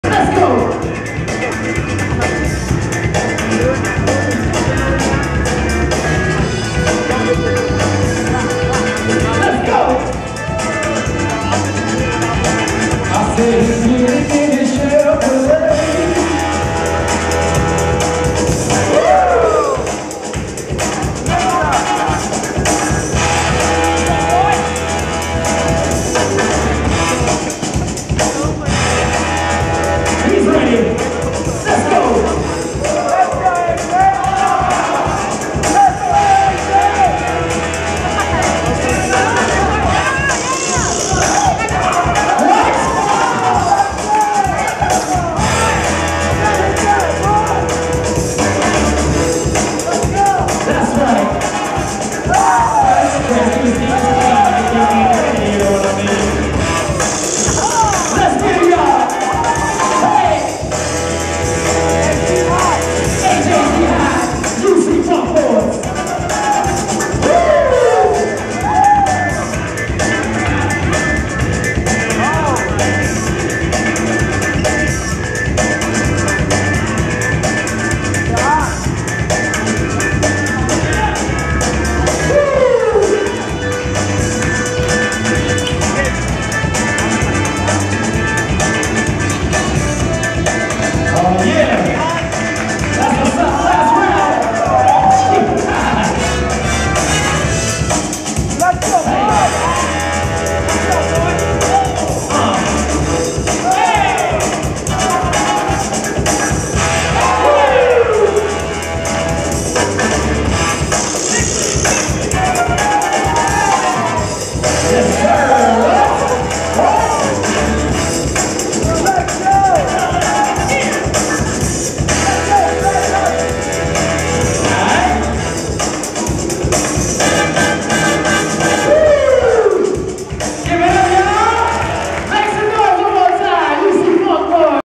2 Fresh two Raw: (Top 16) #6 AJ, Jihad vs. ?
(Top 16) 2v2 B-Boy Battles at 2 Fresh two Raw in San Diego 8/29/15.